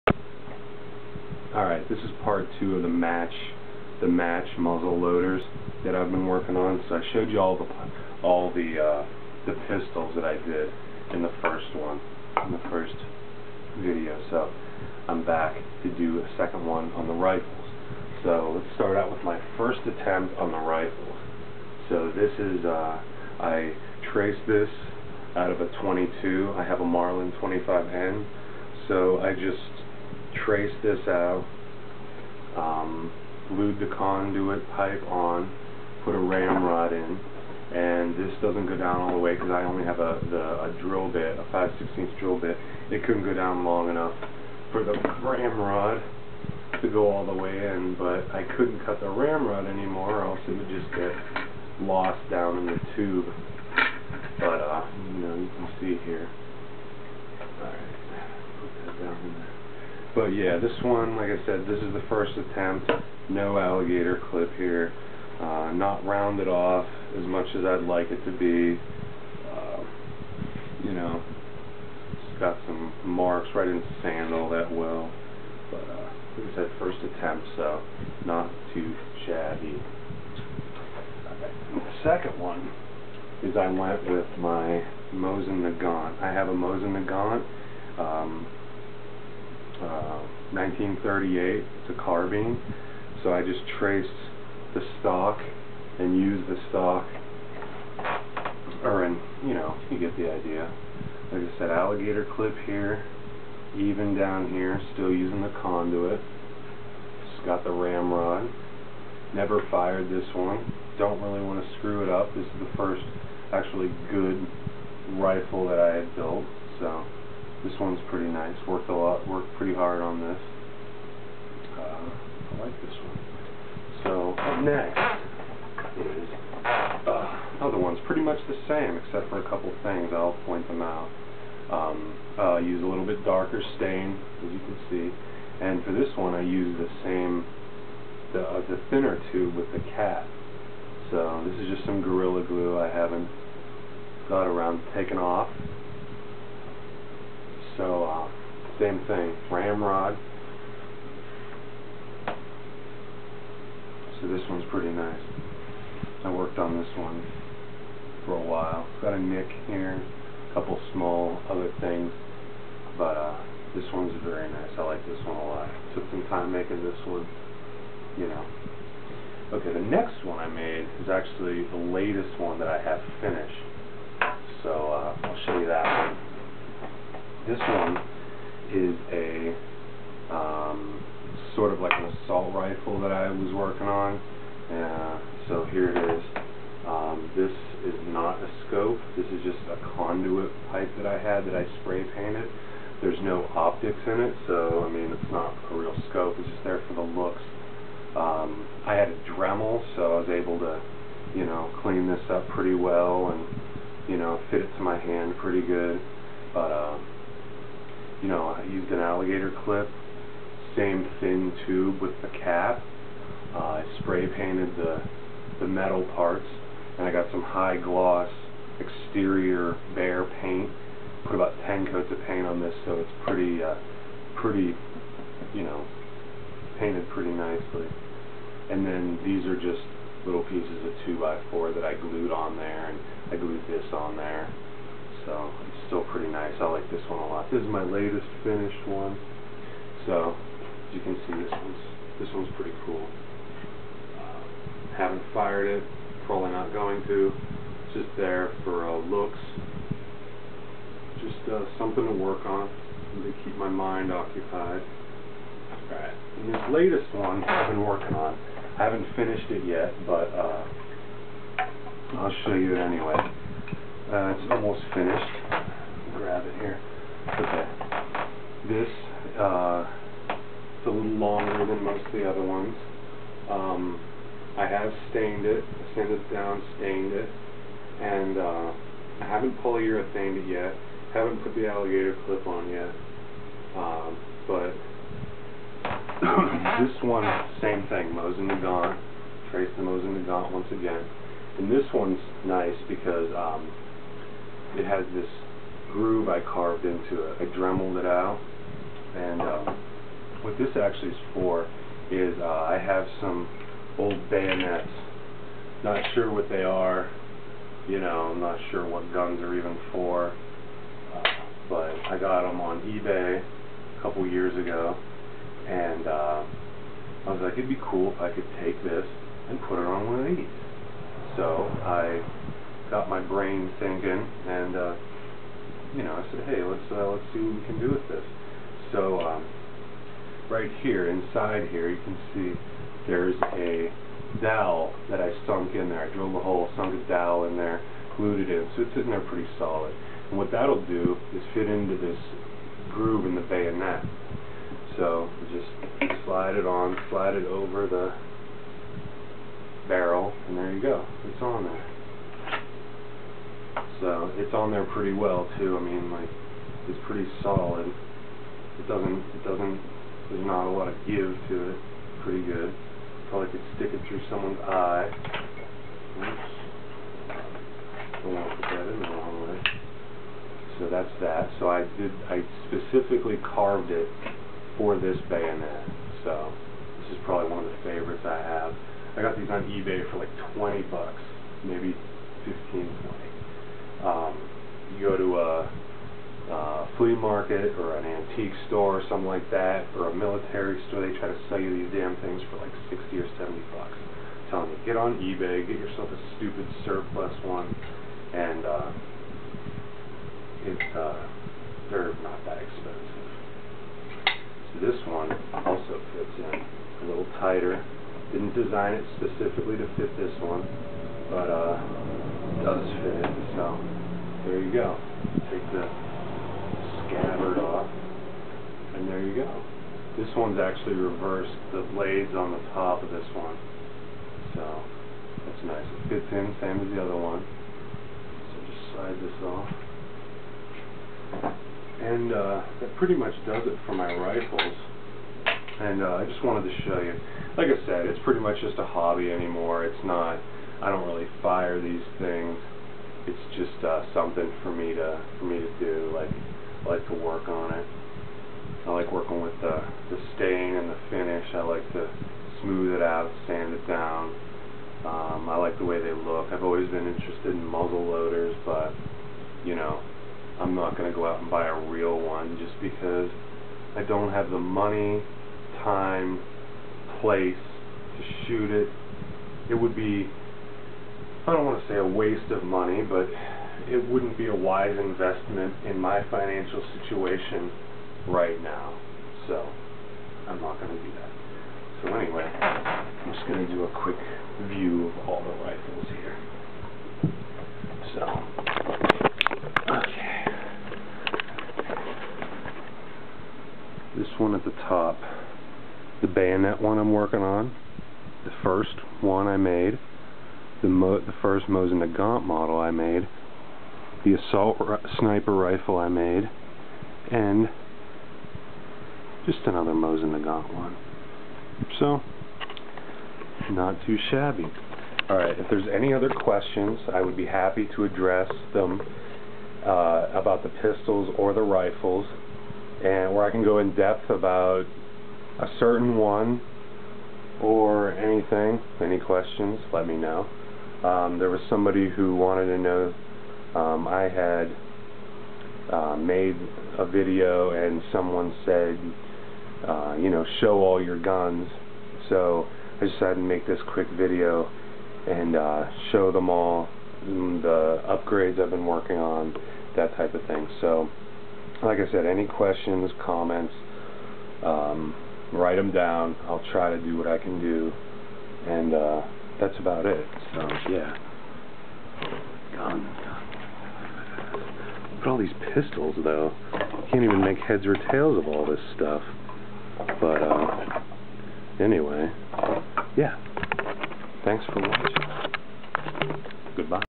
Muzzleloading Match Rifles
An overview of my 5 muzzle loading match rifles.